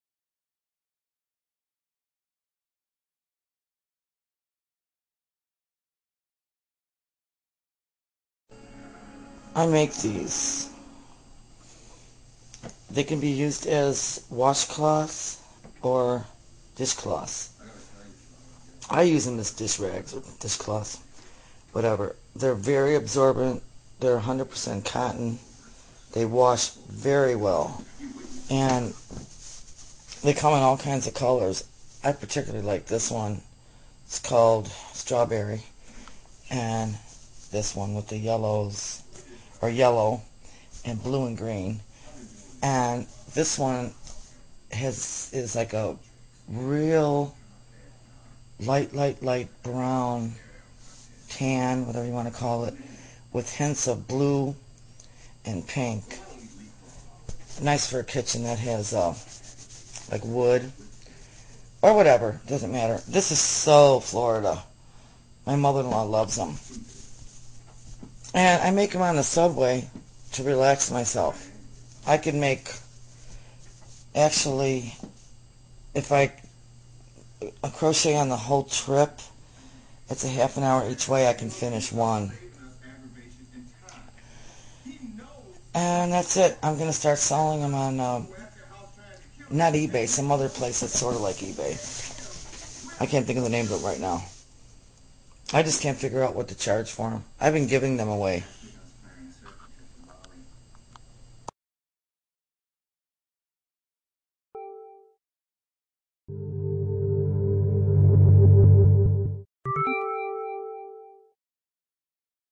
Dish/wash cloths CROCHETED
I used to do these on the "L" on the way to work, it relaxes me and fun to make. but I am retired now. Yarn is expensive, especially this yarn. But, they make nice gifts, stocking stuffers and just something extra when sending gifts to someone.